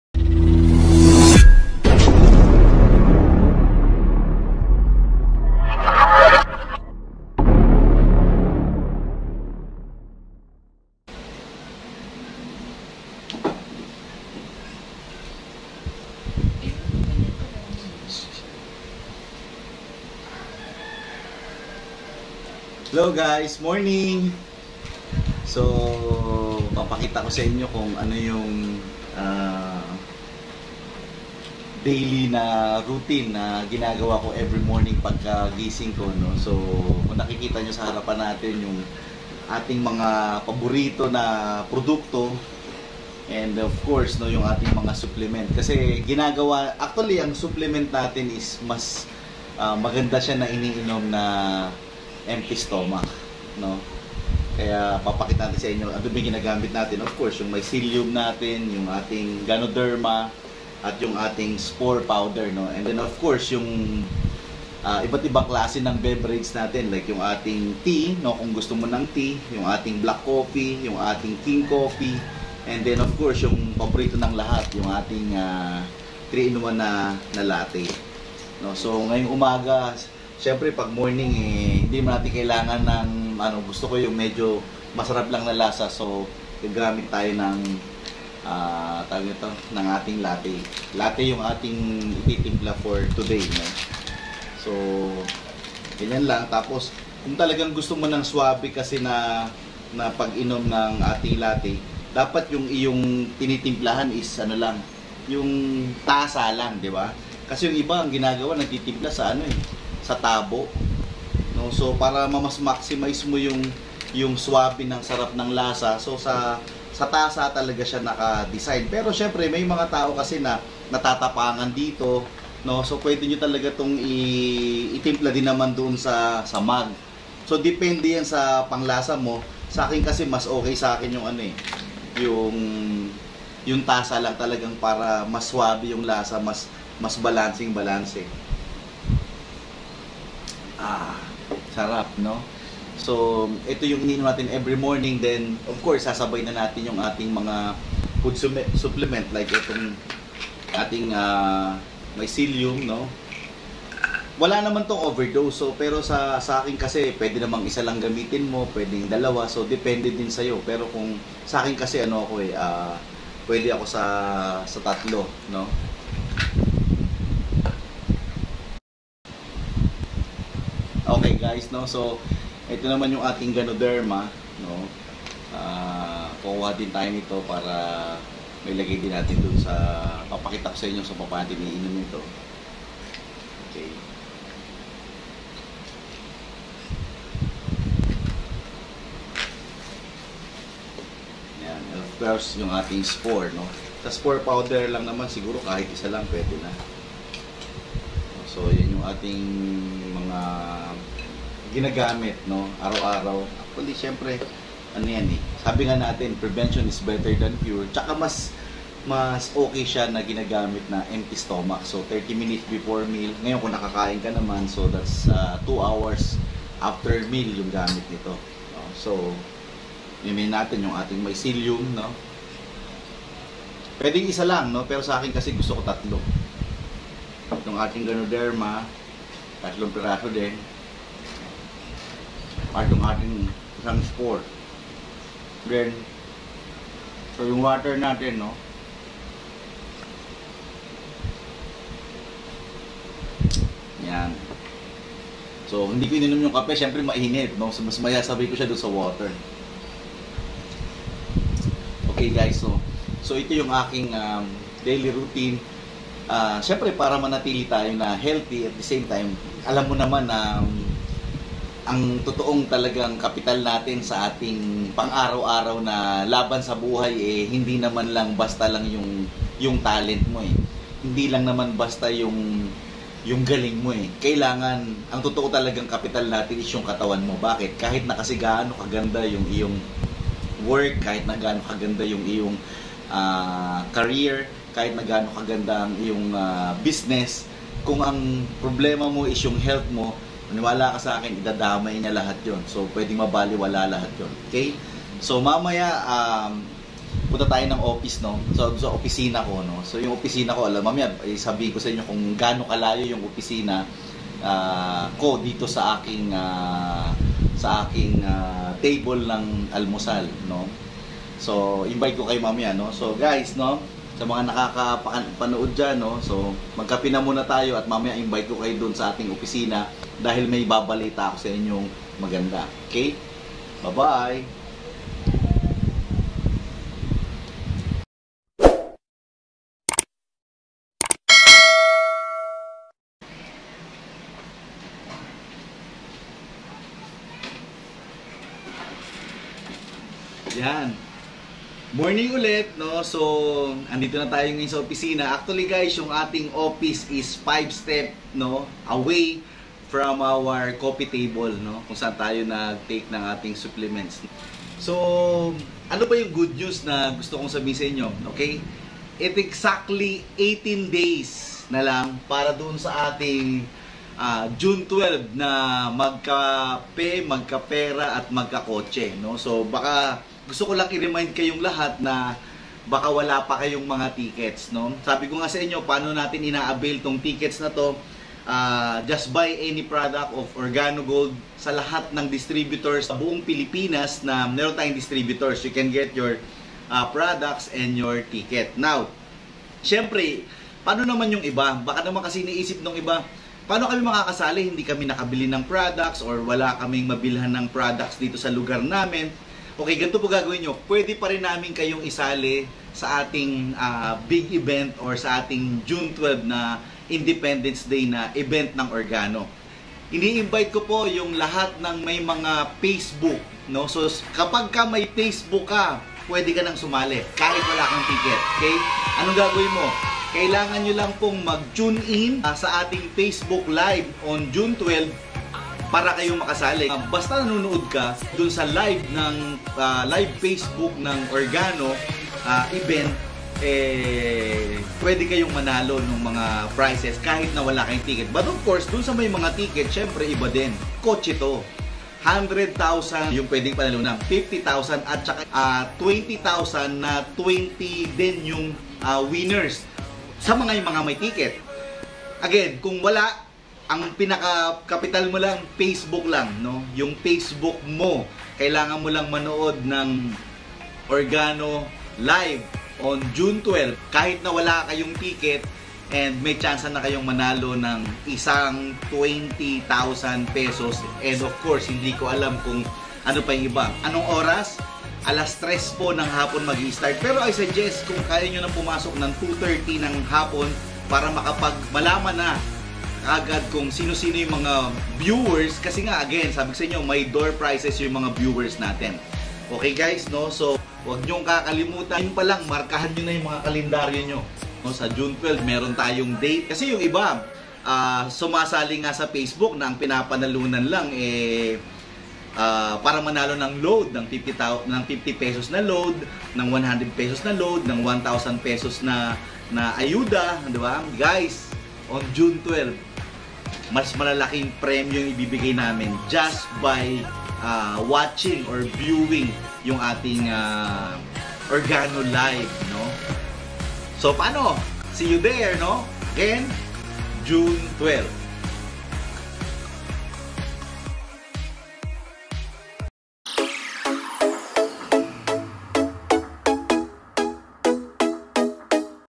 MORNING SUPPLEMENTS ROUTINE
Taking supplements every day is one of my daily routine. since I always believed that prevention is always better than cure...Some food supplements can improve overall health and help control some health conditions. Stay safe, particularly with this herb, which we call THE KING OF HERBS.